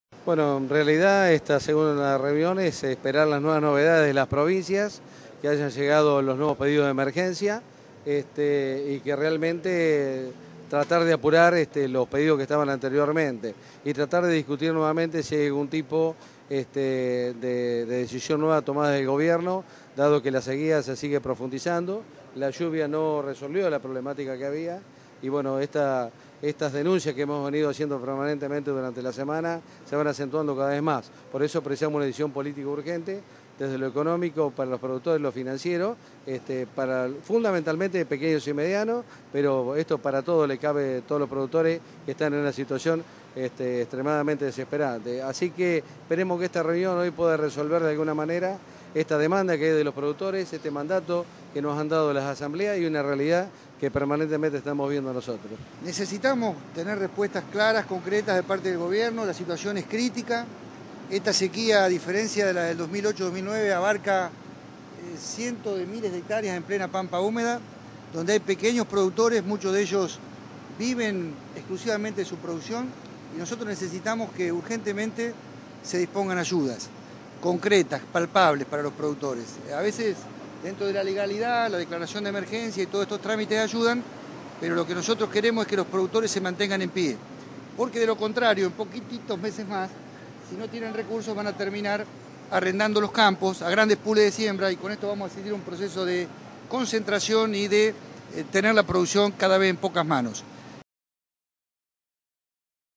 Reunión de gobierno y campo por sequía
El ministro de Agricultura, Norberto Yauhar, se encuentra reunido con
representantes de las principales entidades rurales con el objetivo
de analizar la situación de sequía que afecta a los productores.

Están reunidos en la sede de la cartera el presidente de la
Sociedad Rural Argentina (SRA), Hugo Biolcati; el titular de
Coninagro, Carlos Garetto; el nuevo presidente de CRA, Rubén
Ferrero; y por la Federación Agraria asisten los vicepresidentes
Julián Curras y Guillermo Gianassi.

Curras subrayó la necesidad de "apurar los  pedidos" de ayuda económica, y "discutir si hay algún tipo de  decisión nueva tomada por el Gobierno, ya que la sequía se  profundiza y la lluvia no solucionó la problemática que había".

"Necesitamos una decisión política urgente", aseguró y dijo:
"Esperemos que esta reunión de hoy pueda resolver esta demanda de
los productores".

Detalló que "la zona núcleo, las provincias de Buenos Aires,
La Pampa, Córdoba y Santa Fe están muy afectadas".

Giannassi agregó que "la situación es crítica, los
pronósticos no anuncian lluvias, lo que llovió fue insuficiente y
hay tremendas pérdidas en los cultivos".